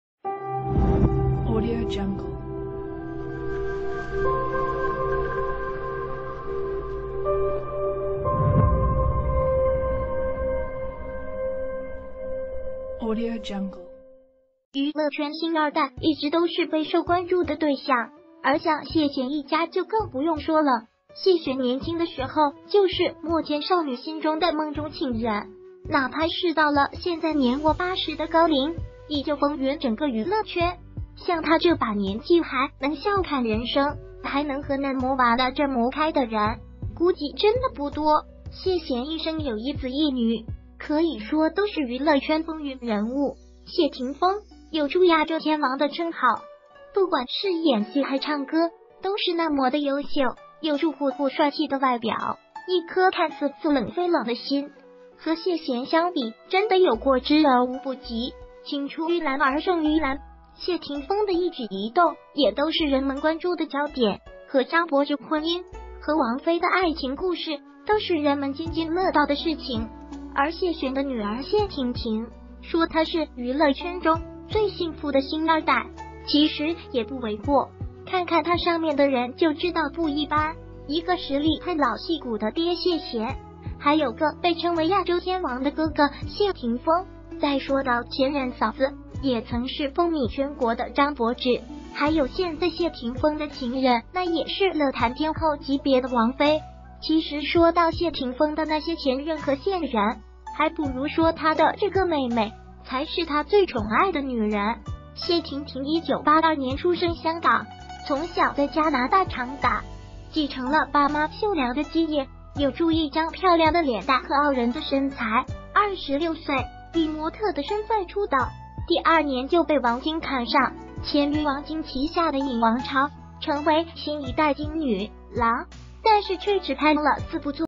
27歲被王晶發掘，之後只演了4部大片，今34歲美如花卻無人敢娶！
娛樂圈星二代一直都是備受關注的對象，而像謝賢一家就更不用說了，謝賢年輕的時候就是萬千少女心中的夢中情人，哪怕是到了現在年過80的高齡，依舊風雲整個娛樂圈。像他這把年紀還能笑看人生，還能和嫩模玩的這麼開的人，估計真的不多。
謝賢一生有一子一女，可以說都是娛樂圈風雲人物。謝霆鋒，有著亞洲天王的稱號，不管是演戲還唱歌都是那麼的優秀，有著酷酷帥氣的外表，一顆看似似冷非冷的心。和謝賢相比真的有過之而無不及，青出於藍而勝於藍。謝霆鋒的一舉一動也都是人們關注的焦點。和張柏芝婚姻，和王菲的愛情故事都是人們津津樂道的事情。而謝賢的女兒謝婷婷，說她是娛樂圈中最幸福的星二代，其實也不為過。看看她上面的人就知道不一般，一個實力派老戲骨的爹謝賢，還有個被稱為亞洲天王的哥哥謝霆鋒，再說到前任嫂子也曾是風靡全國的張柏芝，還有現在謝霆鋒的情人那也是樂壇天后級別的王菲。
其實說到謝霆鋒的那些前任和現任，還不如說她的這個妹妹，才是他最寵愛的女人。謝婷婷1982年出生香港，從小在加拿大長大。繼承了爸媽秀良的基因，有著一張漂亮的臉蛋和傲人的身材。26歲，以模特的身份出道。第二年就被王晶看上，簽約王晶旗下的「影王朝」，成為新一代晶女郎。但是卻只拍了四部作品。